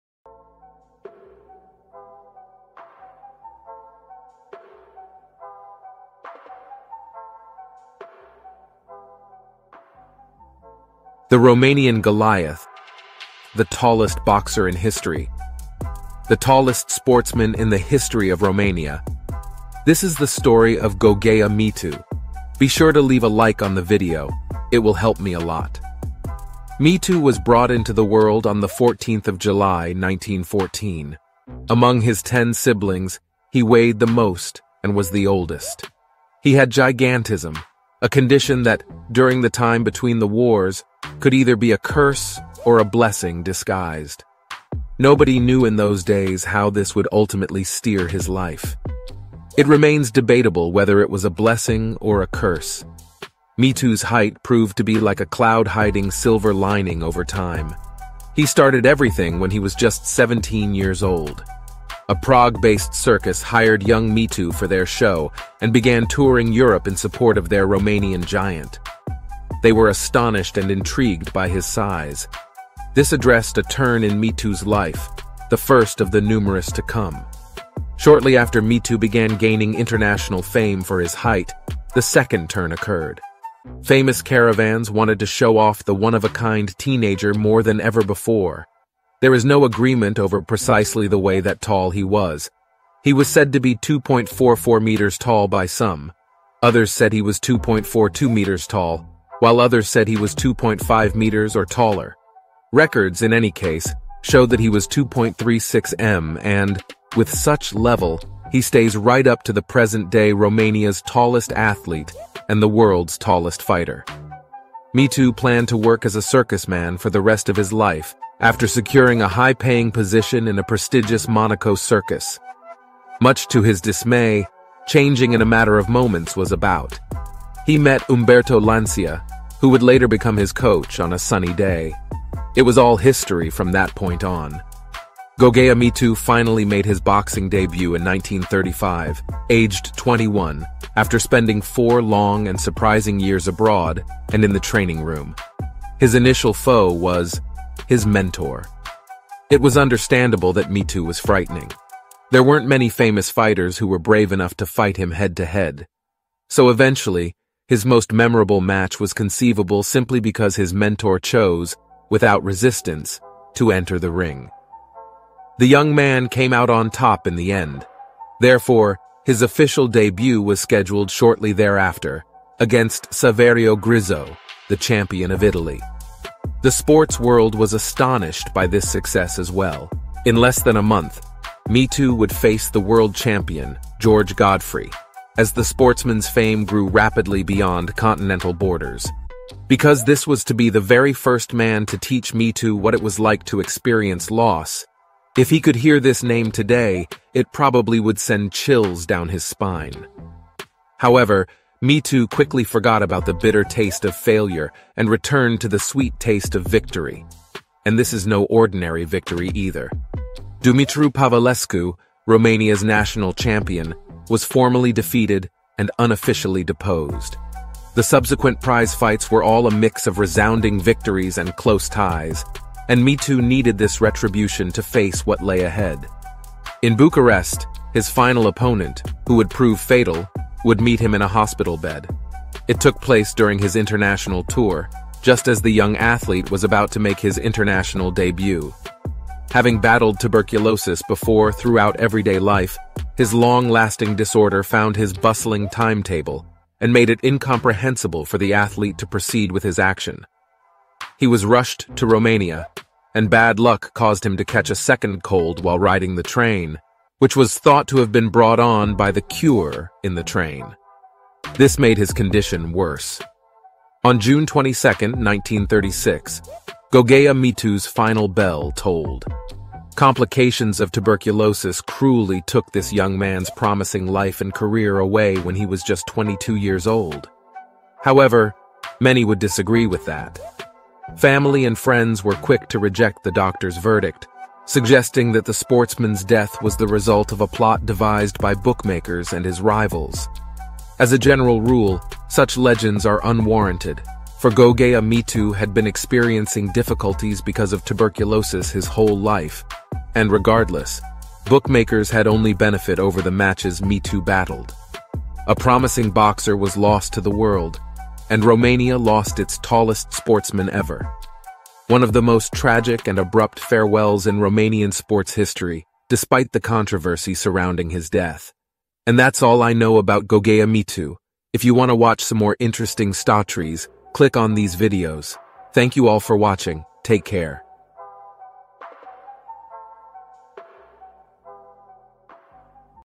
The TRAGIC Story Of The ROMANIAN GOLIATH
we delve into the tragic story of Gogea Mitu, also known as the Romanian Goliath. Join us as we uncover the extraordinary life journey of this remarkable individual, whose story is marked by both triumphs and heartbreak. From his humble beginnings in a small Romanian village to his rise as a celebrated strongman, we'll explore the challenges he faced and the heights of fame he achieved. However, behind the scenes, Gogea Mitu battled personal demons and encountered heartbreaking setbacks that ultimately led to his untimely demise. Through expert storytelling and compelling visuals, we'll shed light on the complexities of his life, the adversities he faced, and the legacy he left behind. Gogea Mitu's story serves as a reminder of the fragility of human existence and the profound impact our choices and circumstances can have. Whether you're fascinated by tales of resilience and tragedy or seeking inspiration from the human spirit, this video offers a poignant exploration of the Romanian Goliath's life. So, join us as we uncover the tragic story of Gogea Mitu, delving into the triumphs, struggles, and ultimate tragedy of this larger-than-life figure. Get ready to be moved by the heartbreaking narrative of the Romanian Goliath in this powerful video.